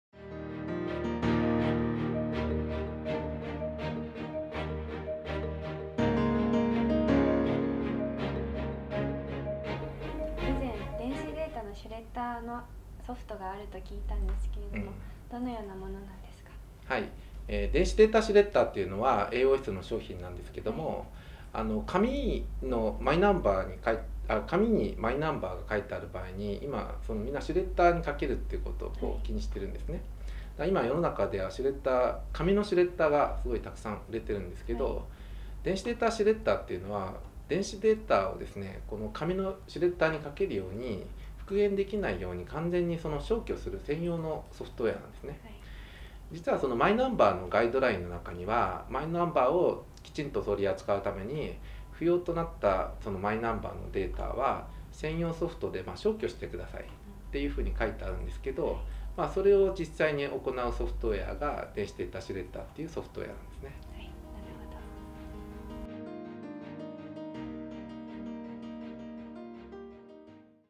マイナンバーQ7「電子データシュレッダーとはどういうソフトですか？」
AOSリーガルテックの佐々木社長に2015年ミス東大の中田茉莉奈さんがインタビューした内容です。
掲載サイトは、こちらです。